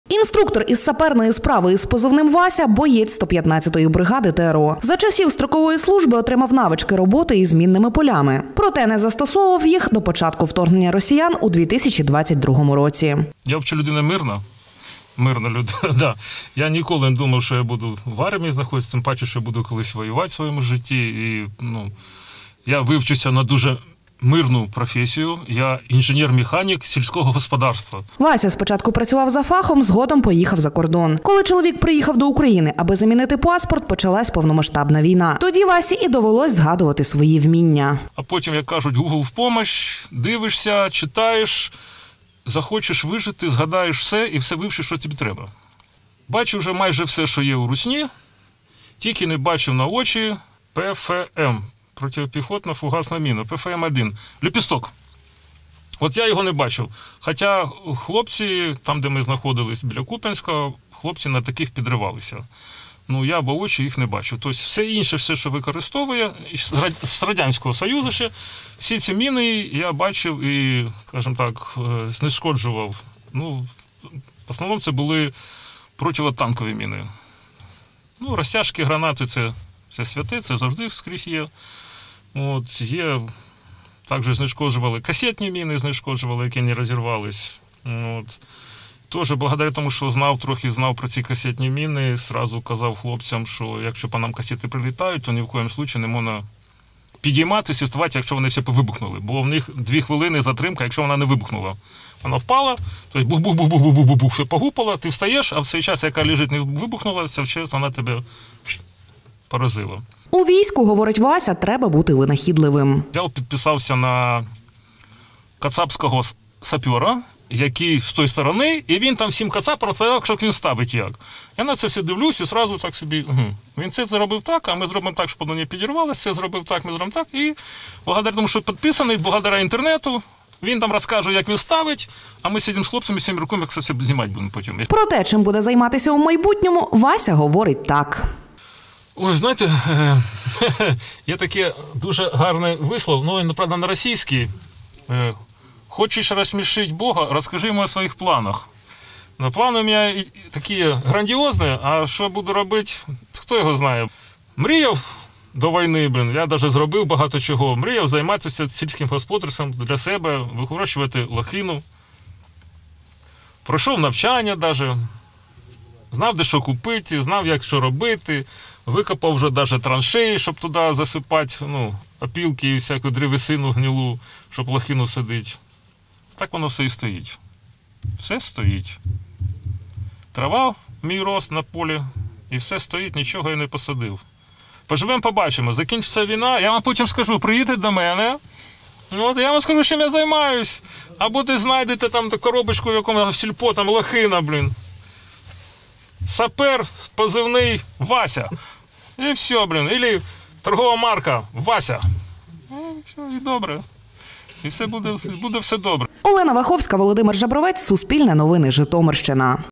«Захочеш вижити — згадаєш усе і все вивчиш, що тобі треба» — інструктор із саперної справи
Інструктор із саперної справи із позивним «Вася» з 115-ї бригади ТрО розповів про те, як потрапив у військо, як в його роботі допомогла підписка на російського сапера та чим планує займатись у майбутньому.